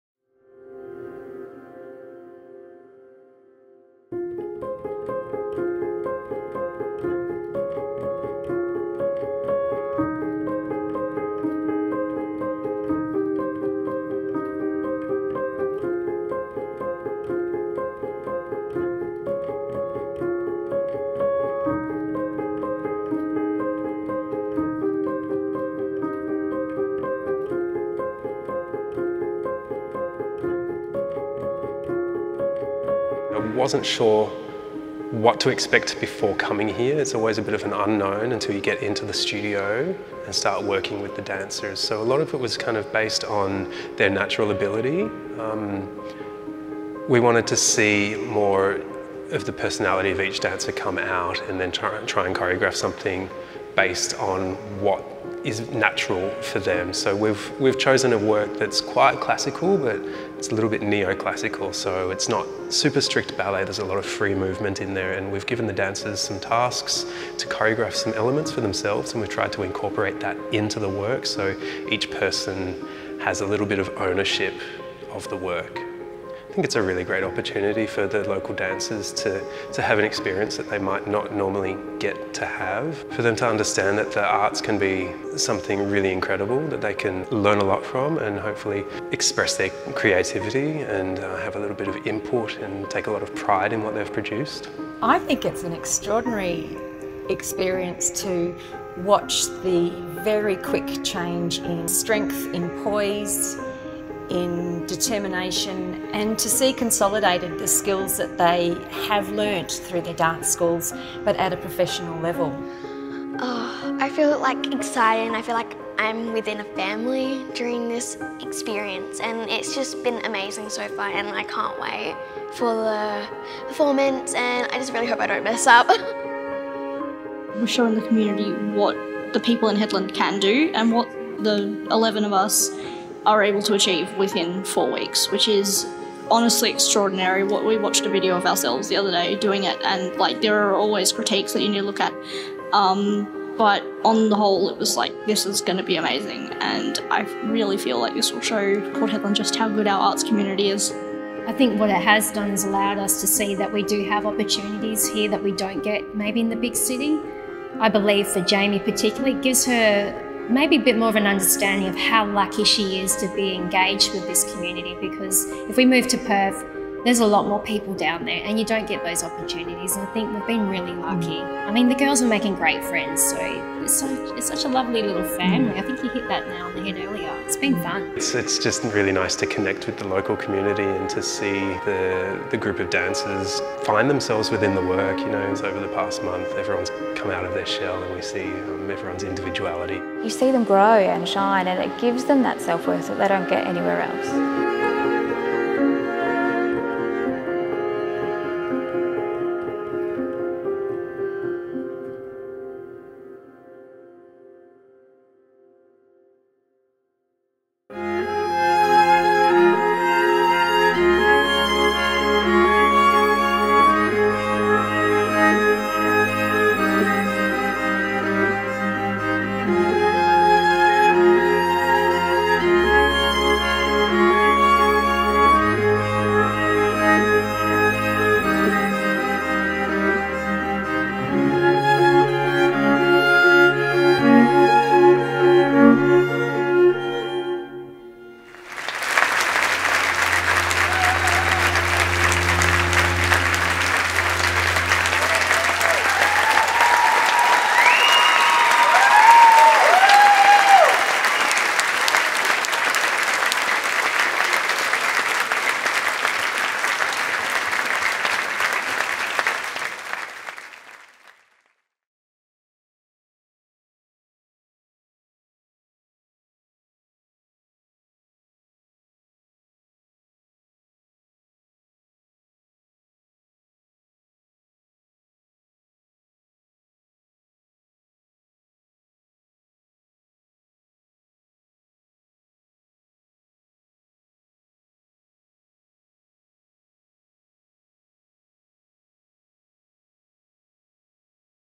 Port Hedland Youth Ensemble | 2019
Port Hedland, on the Pilbara coast in Western Australia is a hub of industry and a vital port for Australia’s resource exports. It’s also the home of hundreds of young people with a passion for dance.

With the support of Roy Hill Community Foundation and the Town of Port Hedland, West Australian Ballet’s Youth Ensemble program was launched in Port Hedland developing skills, increasing capacity, and giving an opportunity for Hedland’s young people to shine.

Spirit
Choreographed by Christopher Hill
Choreographic Assistant: Nicole Ward
Rehearsal Assistant: Tanisha Bolton

Video produced by North West Entertainment Services.